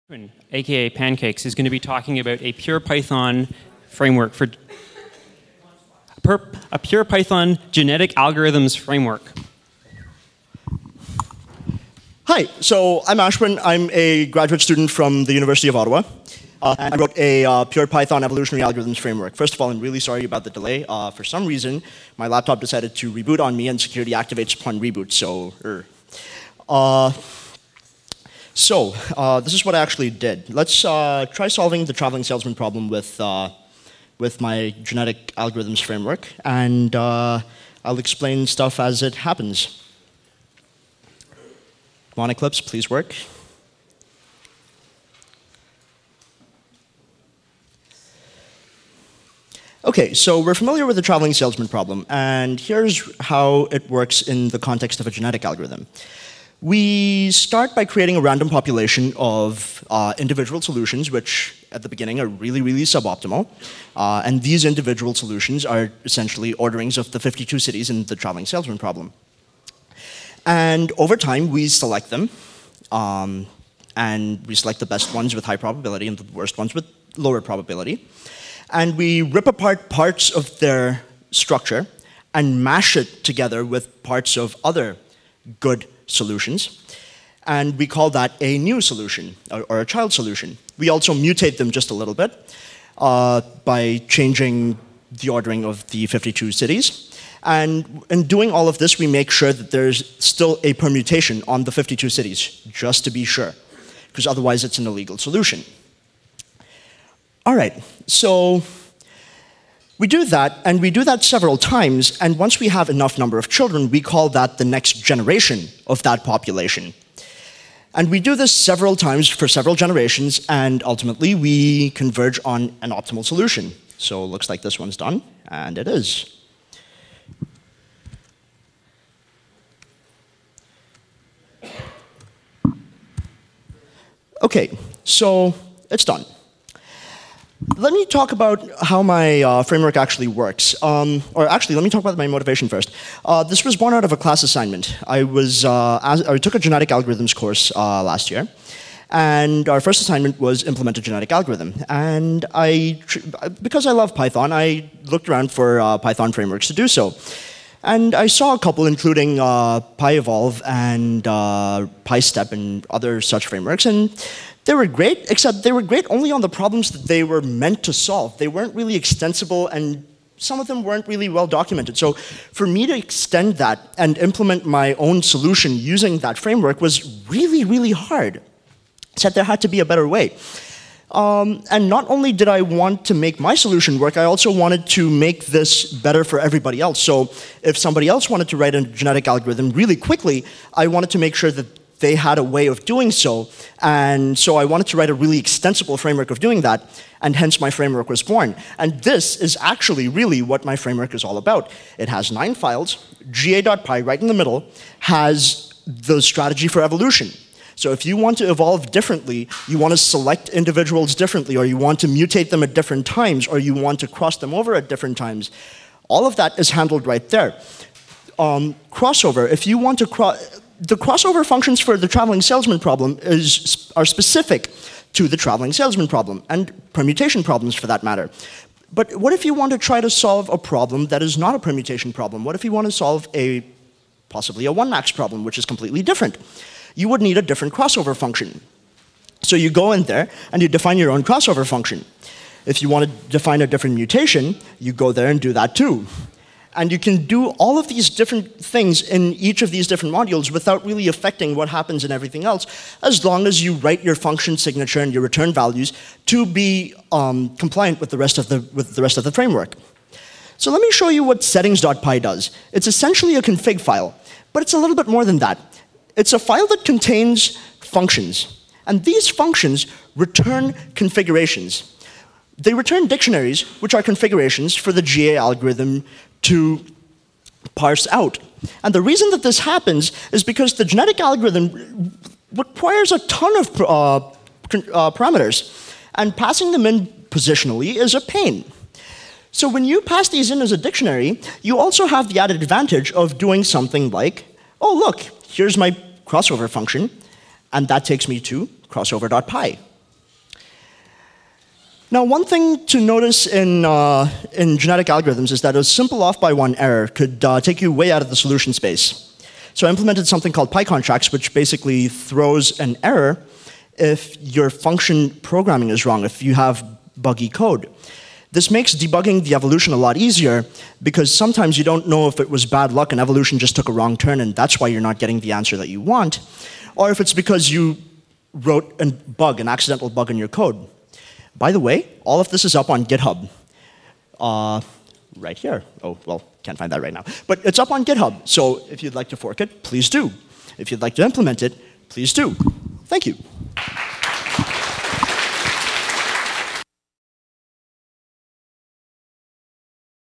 A Pure Python Genetic Algorithms Framework
Ashwin Panchapakesan
Genetic algorithms are a class of evolutionary algorithms, which have been around since the mid 1950s. Since then, a lot of study has been done on these algorithms, using them to solve various different types of problems. While m